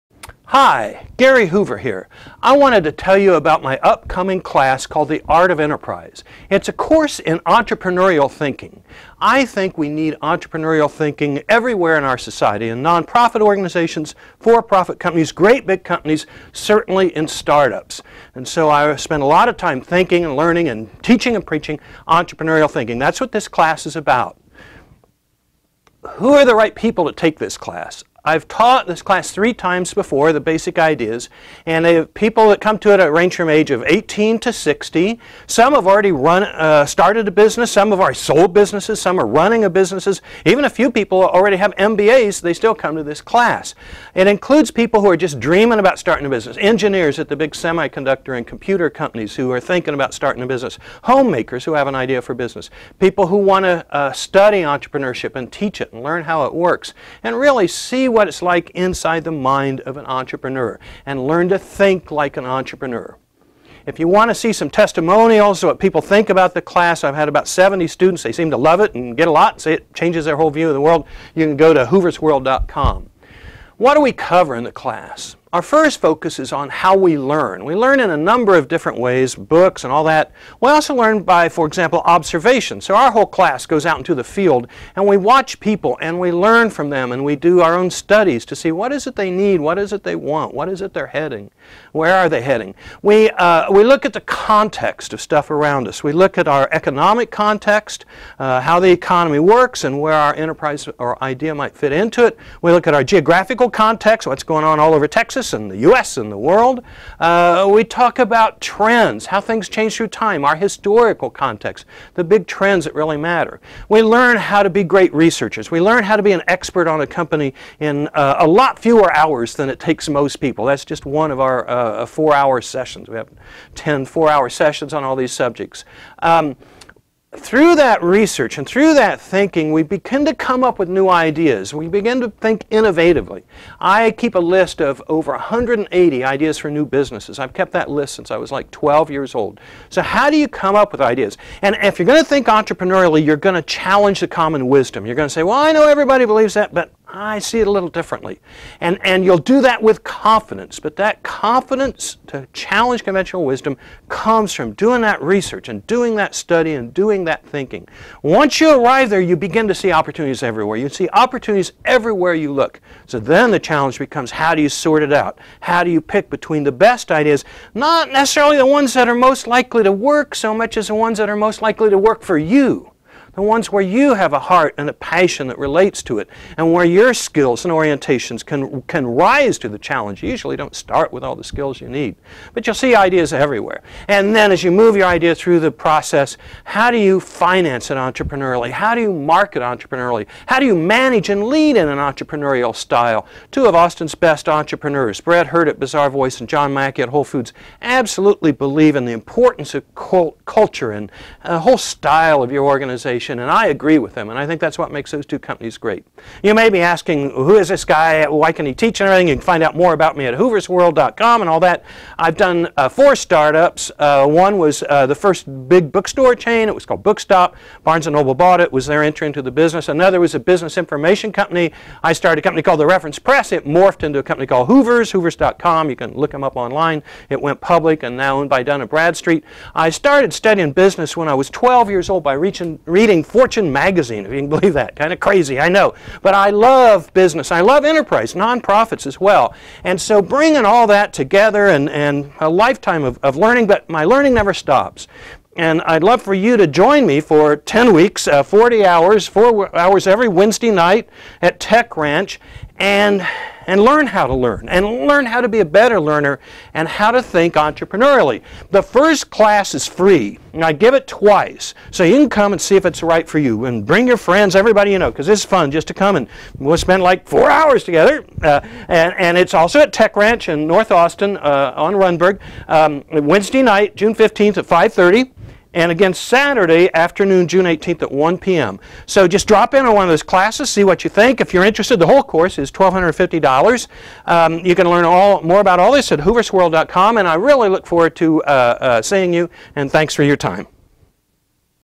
Gary Hoover Art of Enterprise Course
A 5 minute preview of my upcoming course in entrepreneurial thinking.  First class is free, offered twice: Wed. June 15 at 5:30PM and again Sat. June 18 at 1 PM.  That way you can see if the course is right for you.  All classes are at Tech Ranch, 2311 W. Rundberg in North Austin.